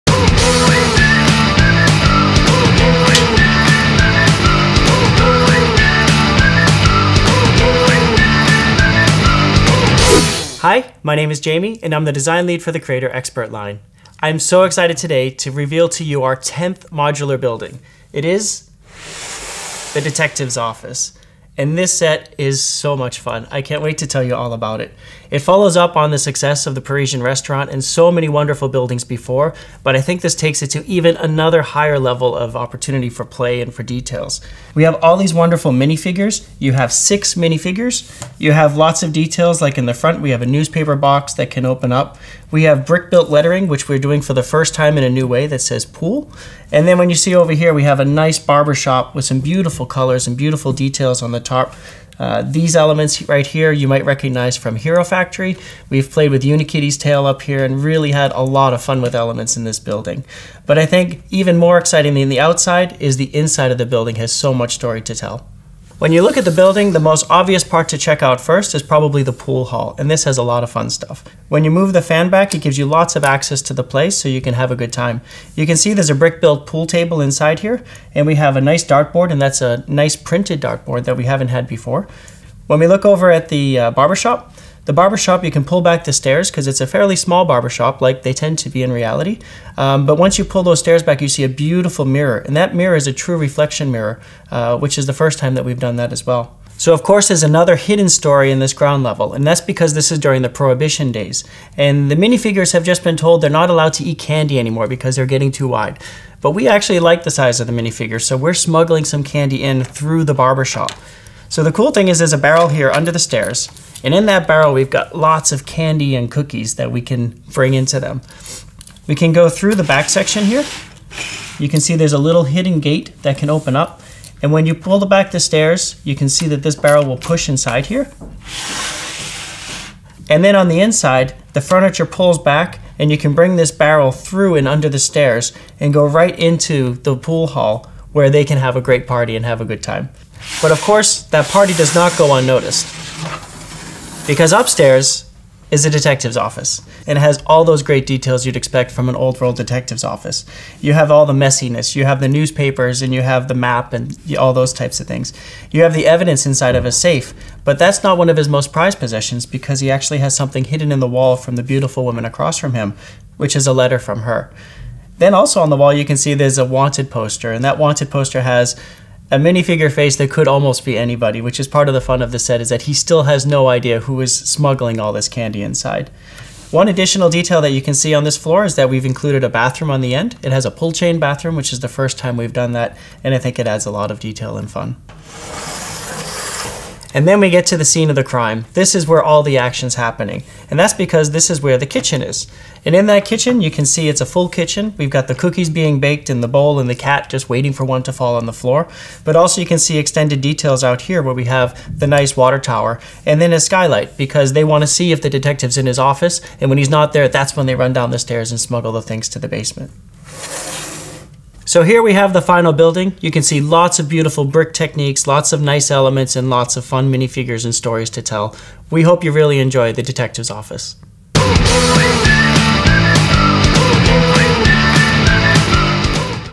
Detective’s Office & Barber Shop - LEGO Creator - 10246 - Designer Video
Discover a world of mystery and adventure with the LEGO® Creator Expert Detective’s Office! Step through the open archway and into the barbershop, where seated customers are pampered in the reflection of a large wall mirror, while next door, competitors play pool and darts beneath the comforting whir of a rotating ceiling fan. Venture to the first floor and you’ll find the detective’s office, his desk strewn with clues, a safe containing valuable evidence and a concealed wall compartment. Then visit the adjacent bathroom, featuring a classic pull-chain toilet, before taking the stairway to the well-equipped kitchen, from where you can access the roof terrace, complete with large water tower. This latest addition to the LEGO Modular Building series is packed with unsurpassed detail and hidden surprises. Easy-to-remove roof and ceilings provide access to the delightful interior, while the exterior of the building features a decorative roofline and a beautifully designed façade. Can you solve the smuggling mystery? Includes 6 minifigures with assorted accessories: Detective Ace Brickman, Al the barber, dart player, pool player, police woman and a mysterious lady in red.

Ages 16+.  2,262 pieces.
US $159.99 - CA $199.99 - DE 149.99€ - UK £132.99 - DK 1399.00 DKK
*Euro pricing varies by country.  Please visit shop.LEGO.com for regional pricing.

Available for sale directly through LEGO® beginning
January 2015 via shop.LEGO.com and LEGO® Stores